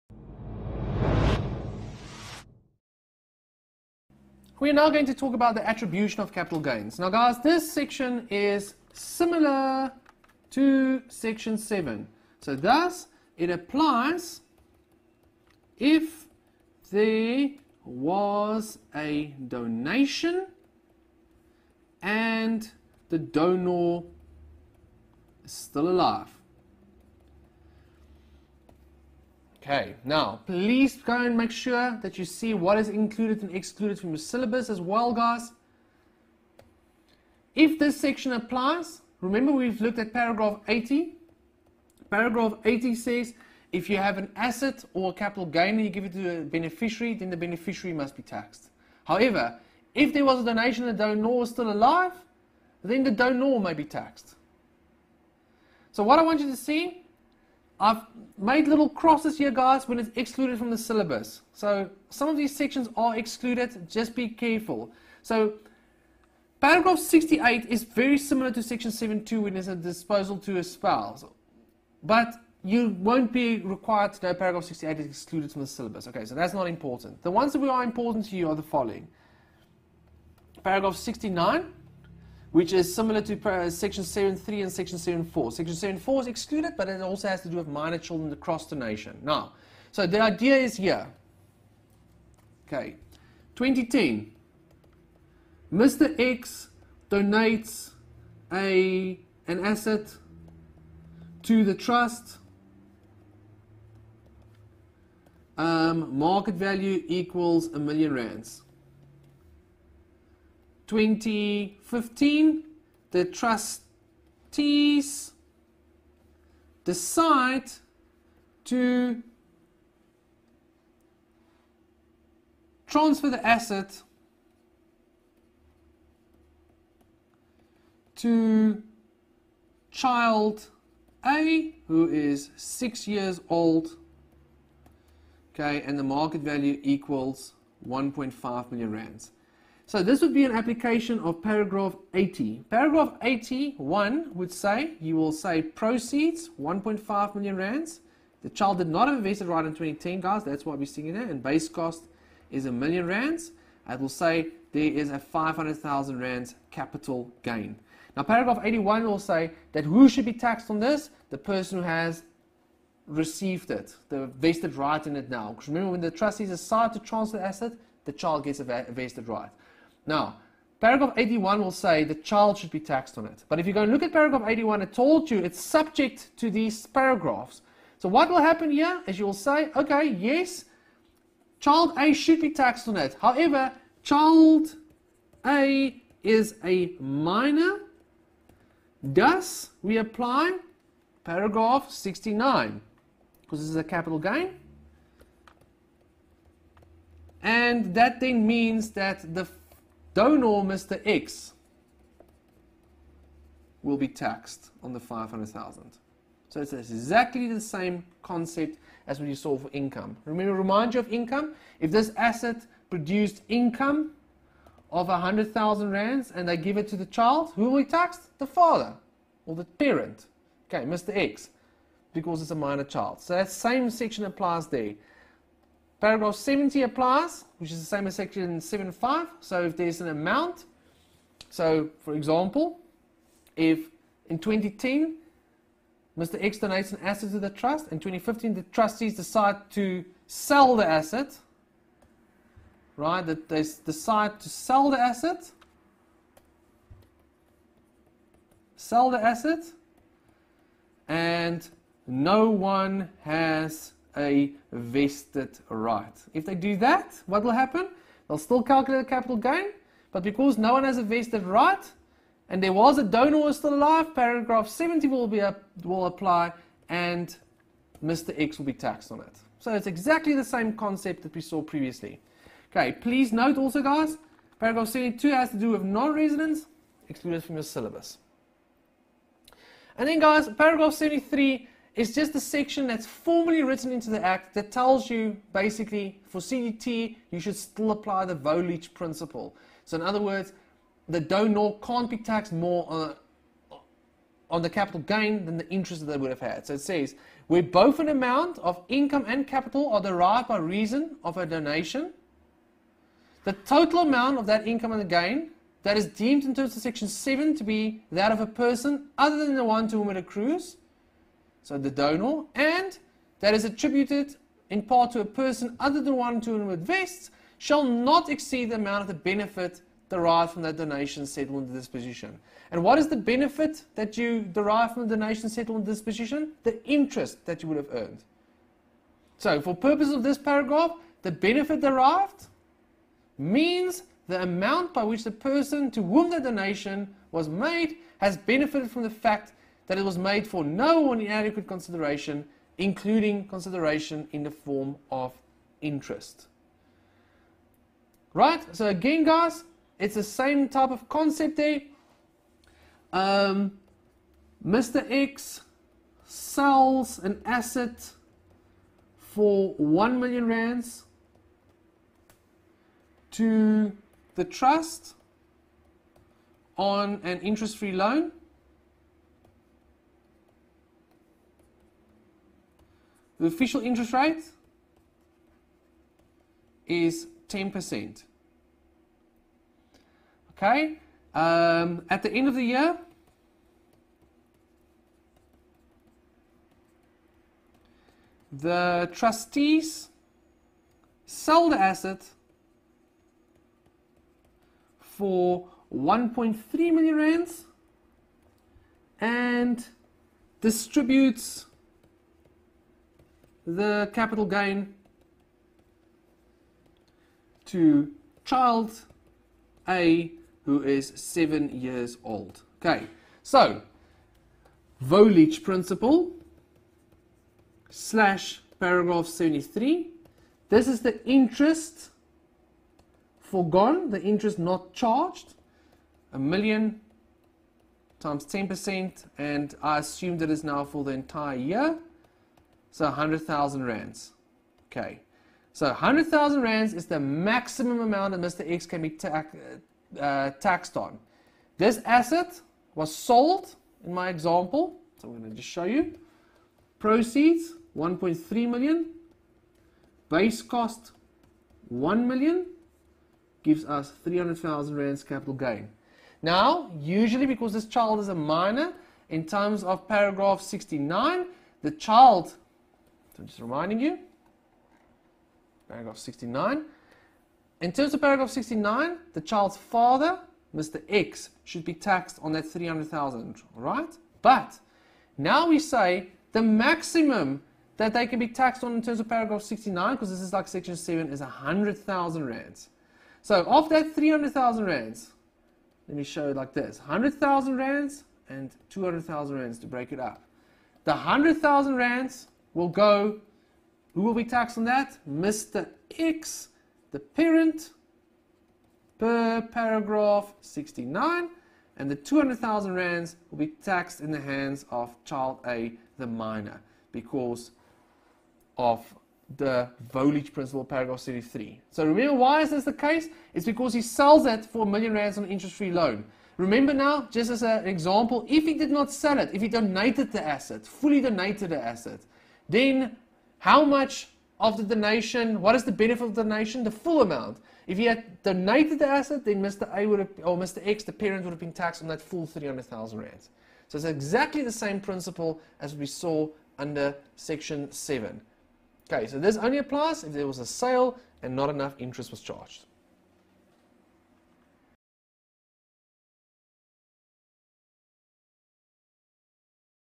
Part 7-Trusts - Attribution of Capital Gains (10min)
In these videos Roulon du Toit CA(SA) works through the Trust principles as per the Income tax act. CTA TAX For all the lecture material, follow this